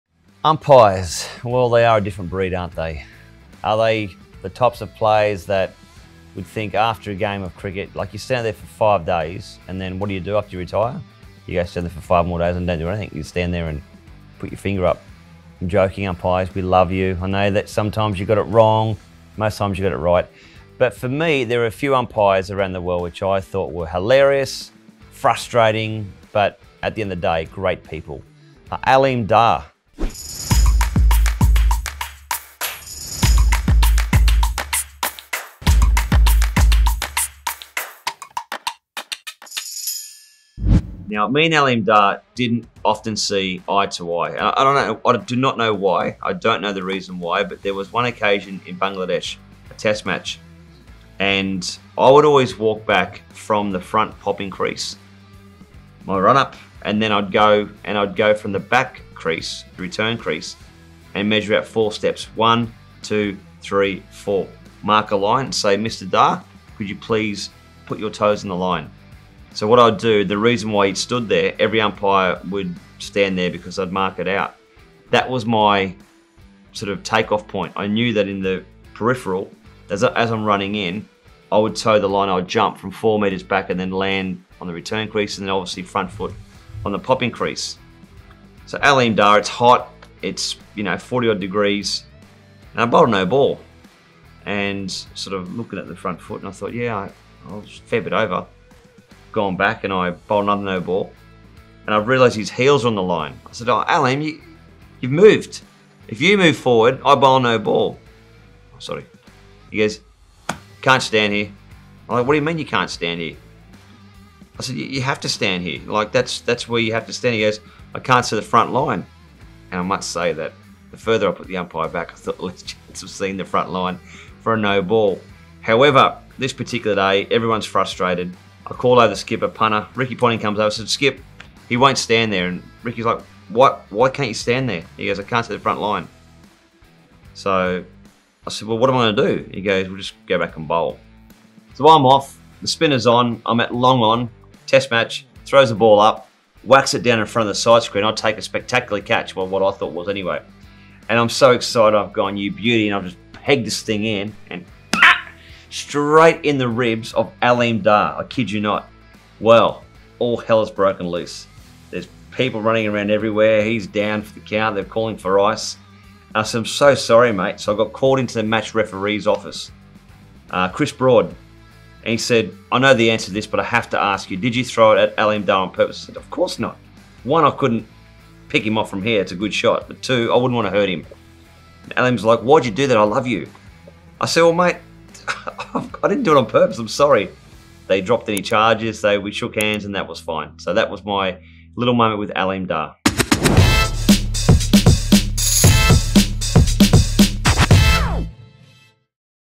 My Oops Moment with Aleem Dar I Umpires I Brett Lee TV
Here's the story of my "Oops moment" with Umpire Aleem Dar!